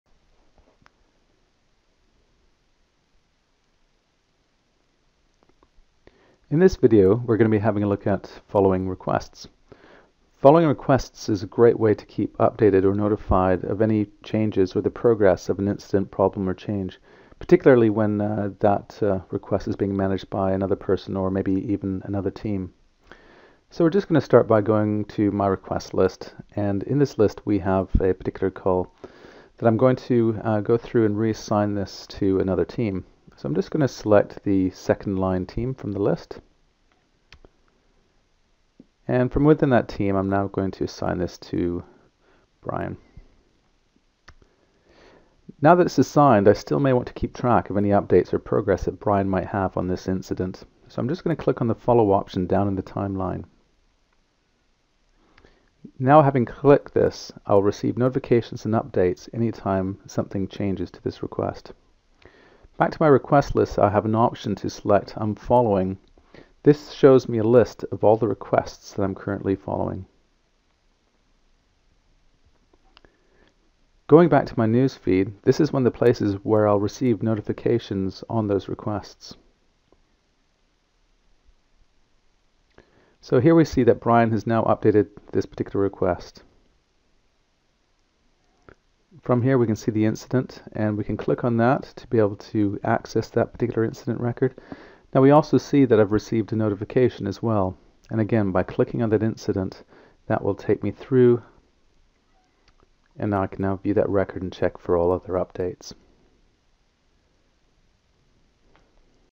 Following Requests - Feature Friday Blog, Hornbill
This week's Feature Friday video takes a look at following requests in Hornbill Service Manager.

Following requests lets you keep up-to-date with the progress of requests that are important to you. This is a great feature for managers to keep on top of what is happening on high profile or major incidents.

As updates are made to a request, these are made visible on the follower's Newsfeed.  Followers also receive a notification to alert them of the update. Whether they are viewing Hornbill from their computer or using the Hornbill Mobile App, they will always be made aware of the latest updates.